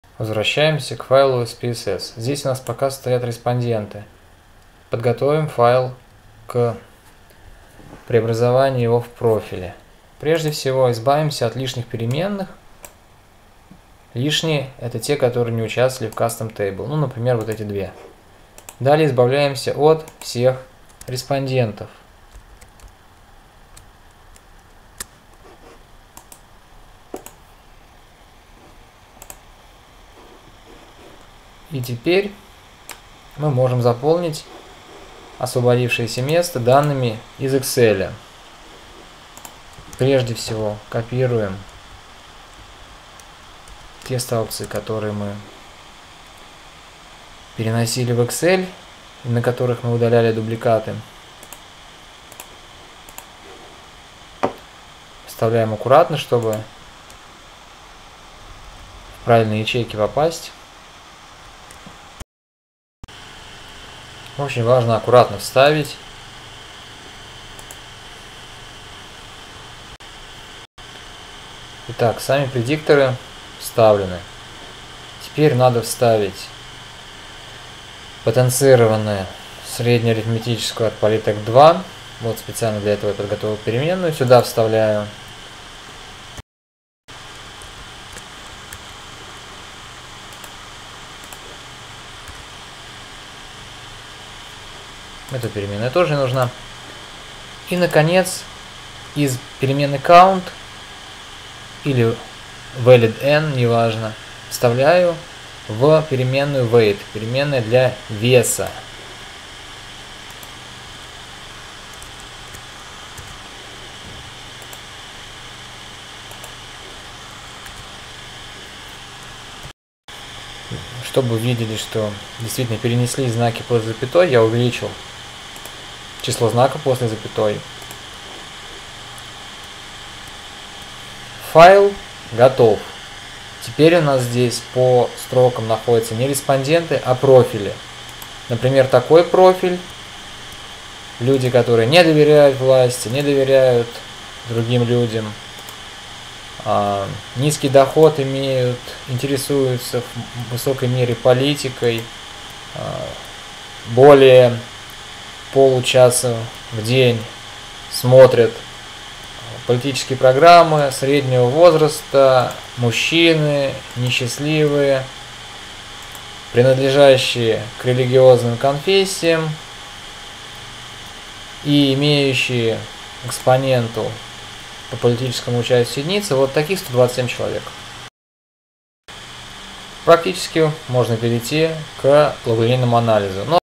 Точечно-линейная регрессия 8
Непосредственное создание файла с профилями.
Навигация по фрагментам видео (после перехода используйте область навигации по странице справа)